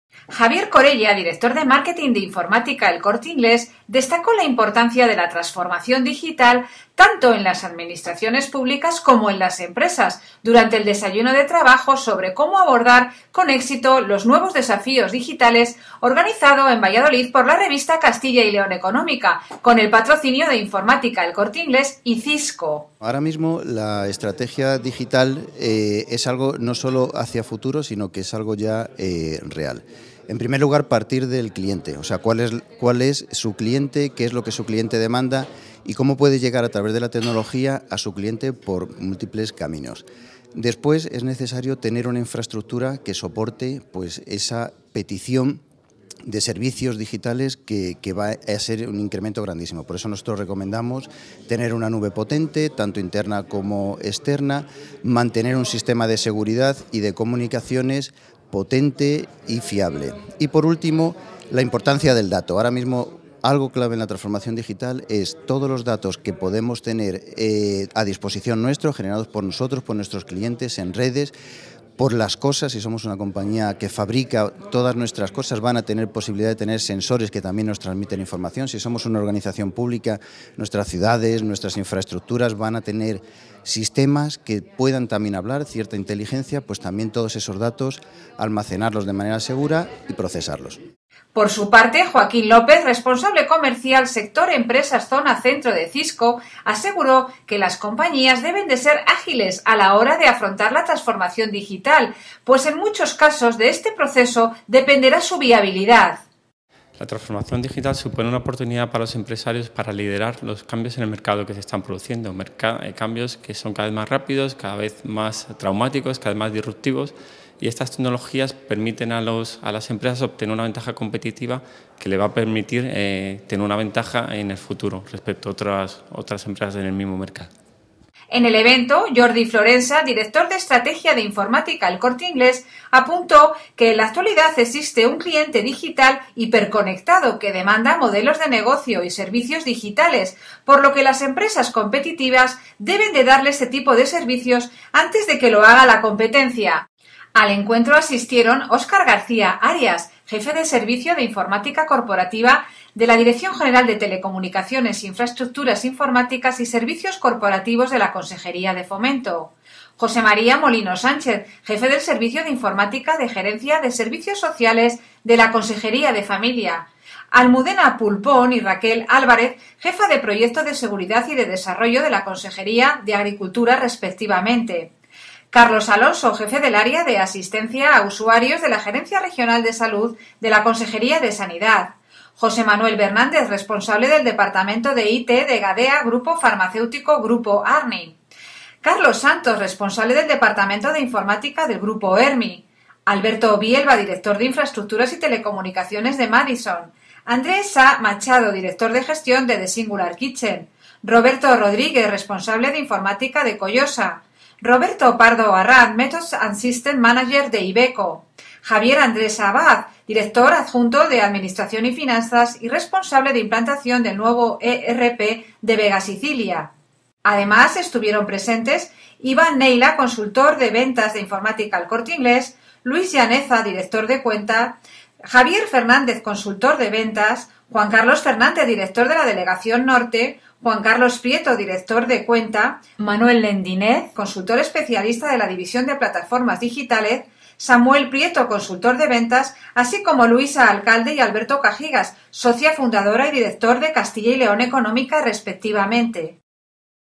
Desayuno de trabajo: Cómo abordar con éxito los nuevos desafíos digitales
Javier Corella, director de Marketing de Informática El Corte Inglés, destacó la importancia de la transformación digital tanto en las administraciones públicas como en las empresas, durante el desayuno de trabajo 'Cómo abordar con éxito los nuevos desafíos digitales', organizado en Valladolid por la revista Castilla y León Económica con el patrocinio de Informática El Corte Inglés y Cisco.